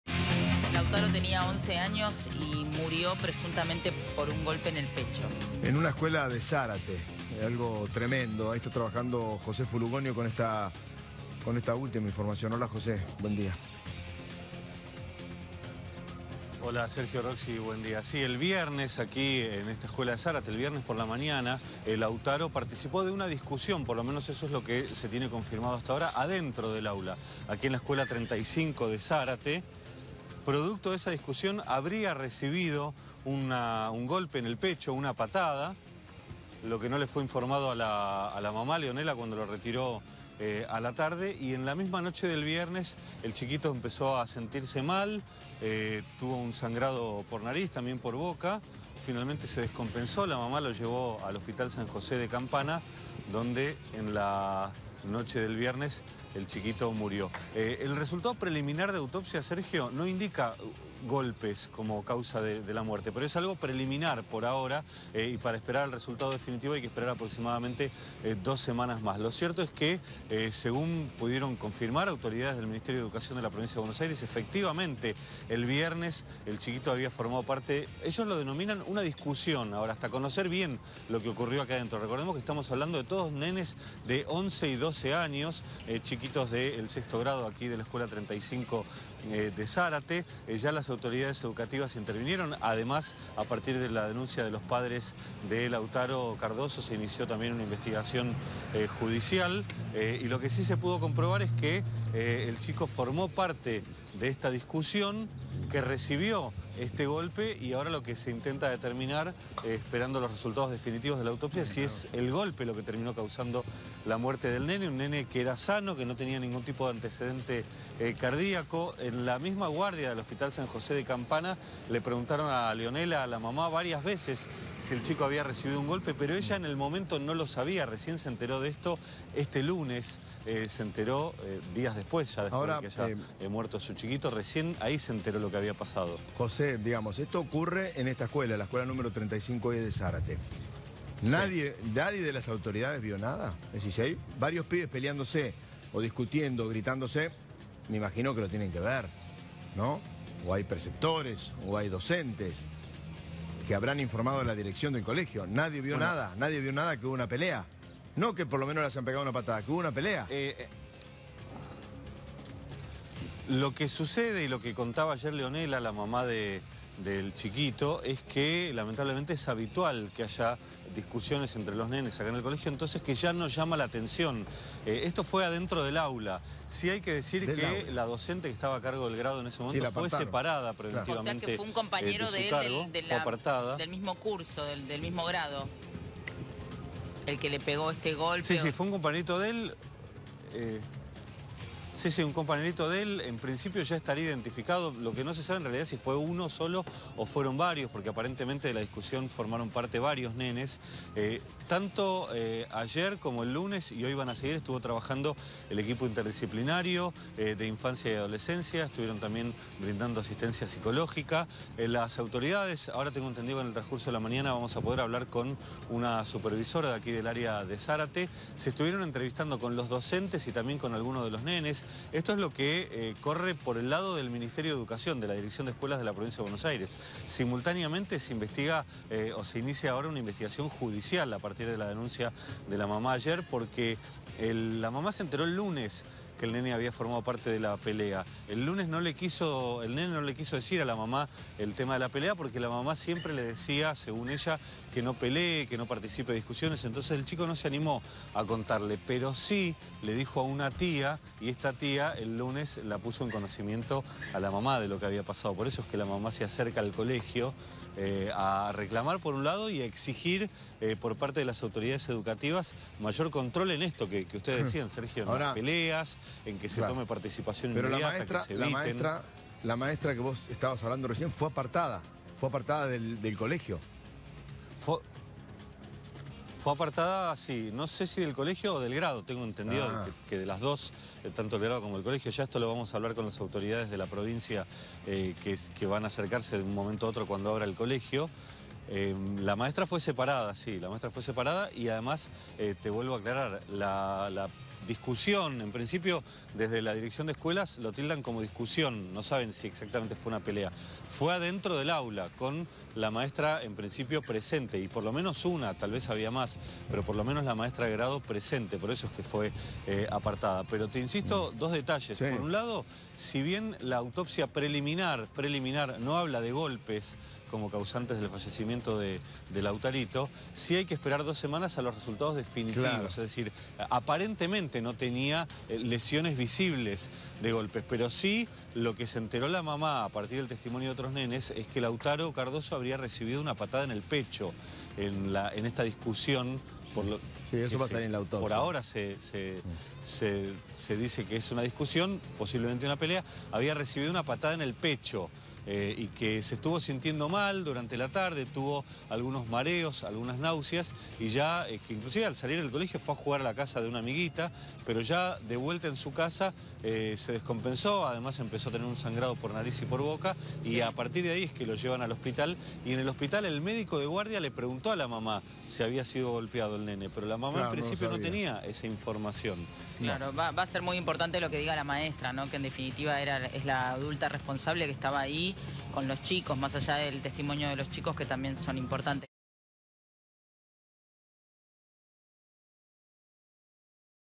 Dramático: a un nene de 11 años le pegaron en la escuela y murió horas después
Impactante caso de violencia escolar en Zárate. Lautaro Cardozo tenía 11 años Iba a la escuela en Zárte. Participó de una discusión adentro del aula en la escuela 35 y en medio de esa pelea recibió una patada. Nadie se lo informó a su mamá, Leonela. Esa misma noche se sintió mal, se descompensó y murió en el hospital.
Se ordenó una autopsia, que no habla de golpes, pero la maestra que estaba a cargo del grado fue apartada de su cargo.